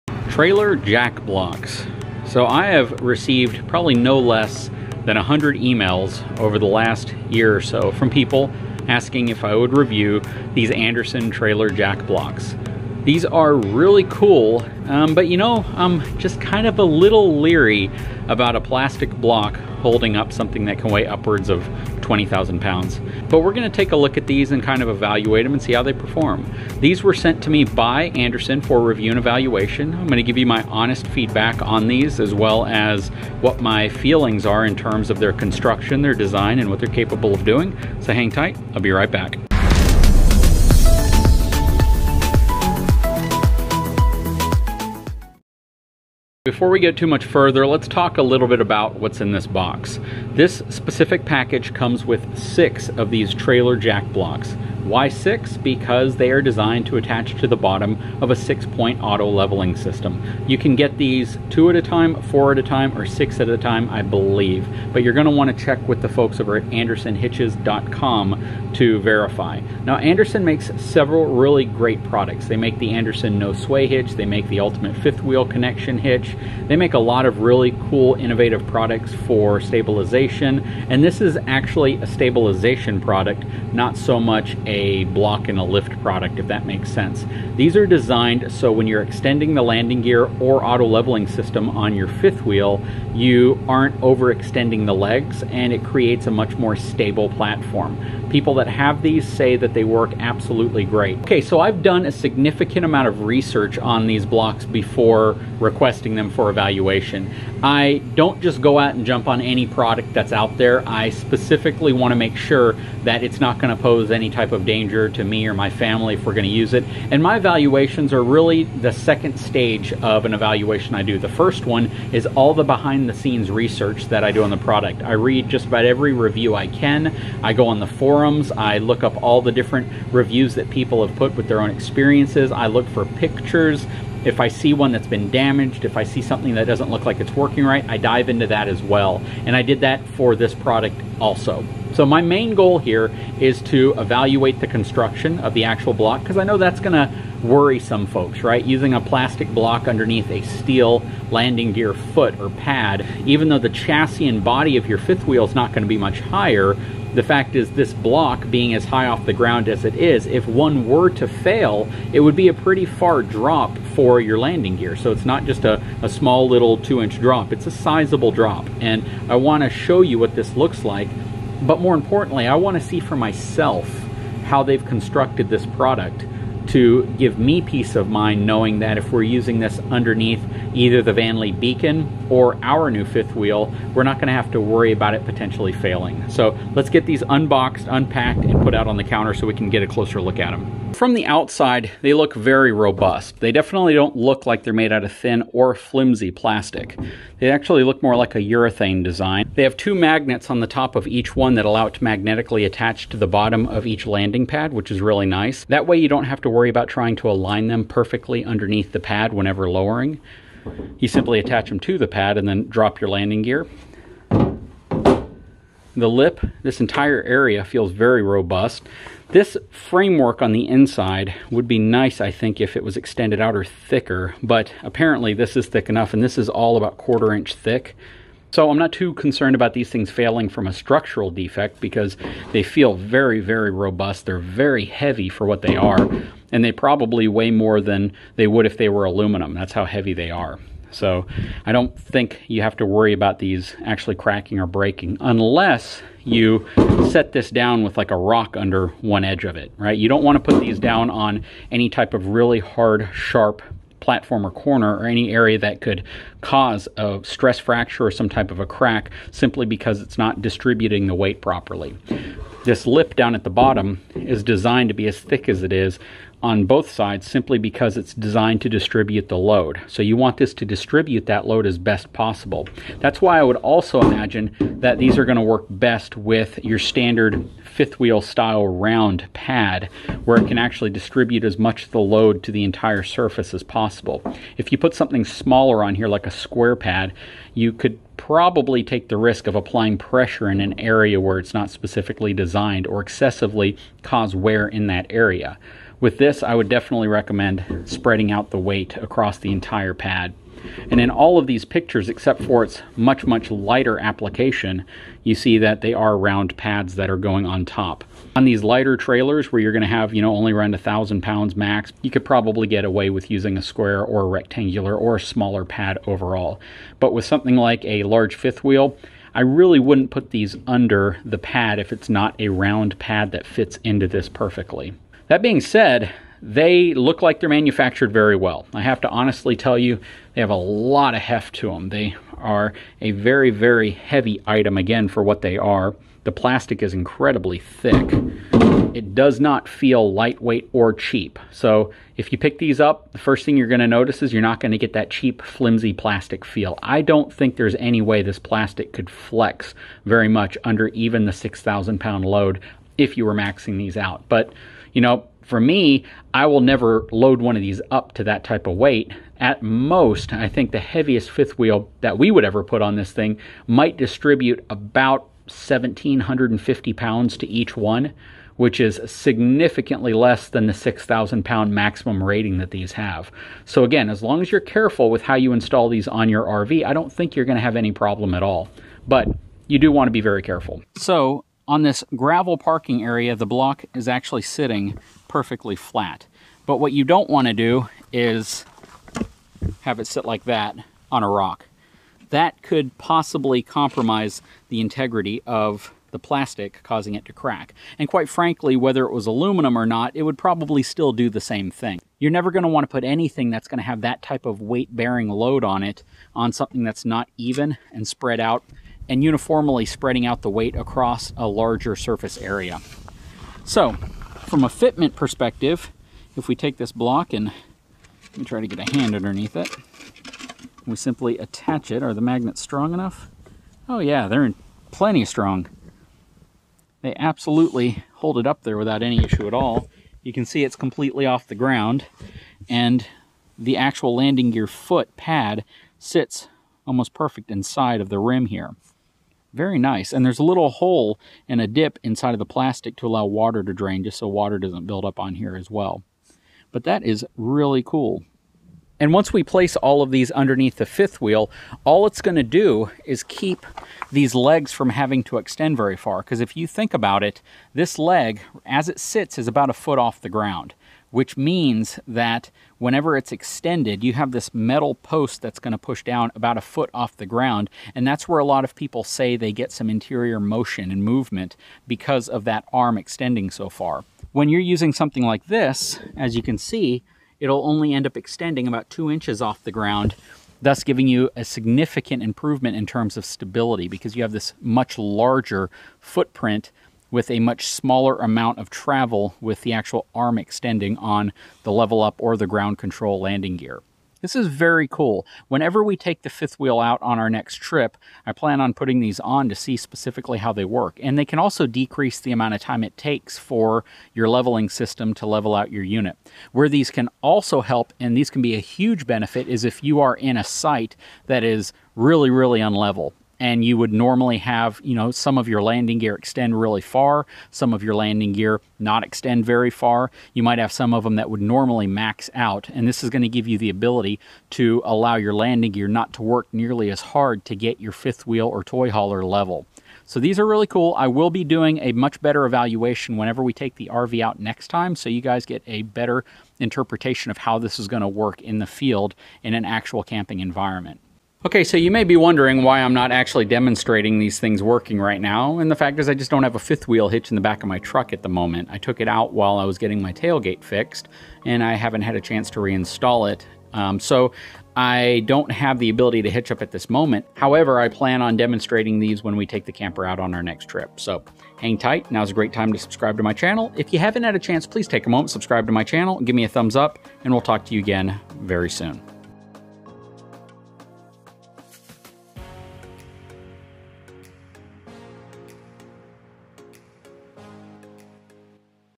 These PLASTIC RV Stabilizer Jack Blocks are cool! Andersen Jack Blocks
Andersen Jack Blocks

My wife's RV Channel! Mrs BTBRV

Link to all the accessories I review and use from etrailer!